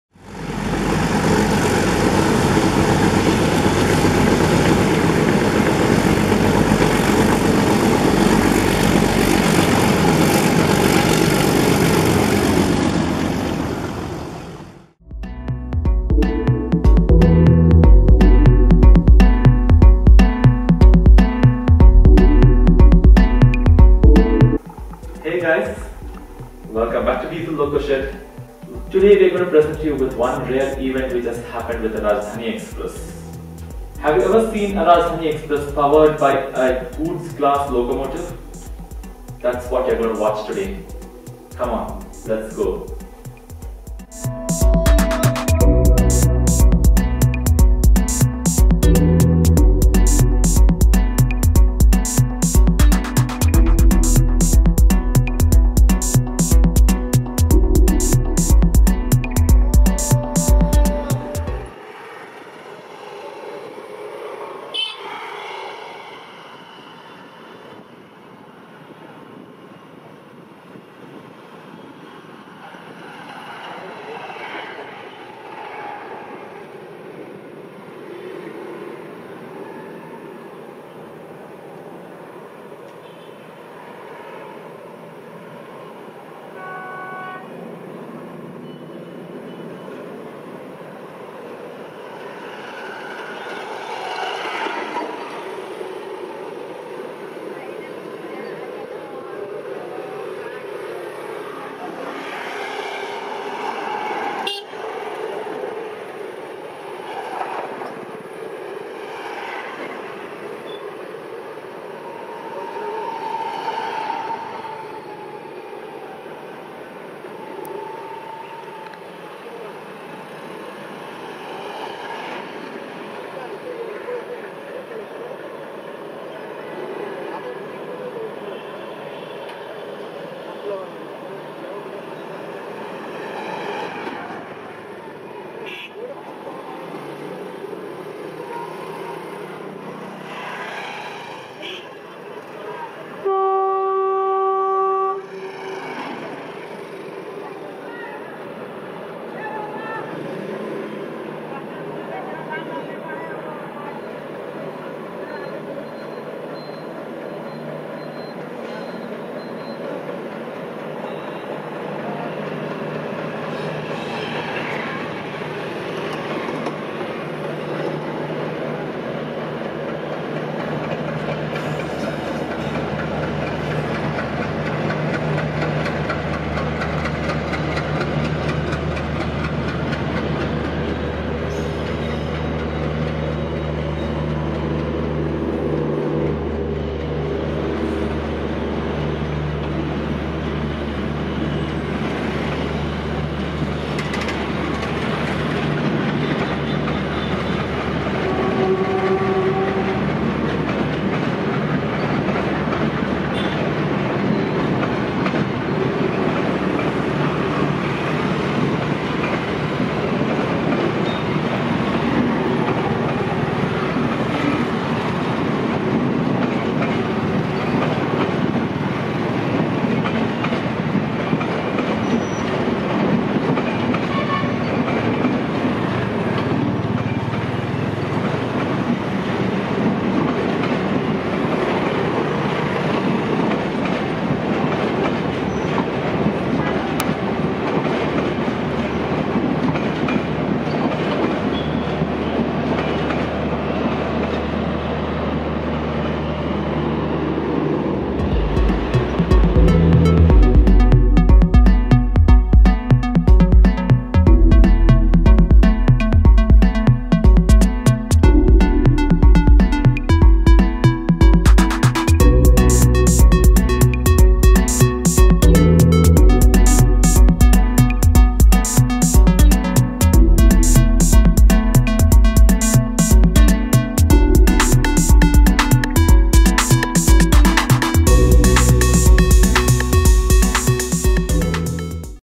RAREST of RARE !! ERS ALCo with TVC RAJDHANI EXPRESS | Indian Railways
Diesel Loco Shed presents a Very Rare Event, featuring the legendary Thiruvananthapuram Nizamuddin Rajdhani Express (12431) !! The Train, powered by a WDG 3A ALCo from Ernakulam departs after a scheduled halt at Madgaon, switching tracks and snaking away !! The Train was running late by more than 2.5 hours !!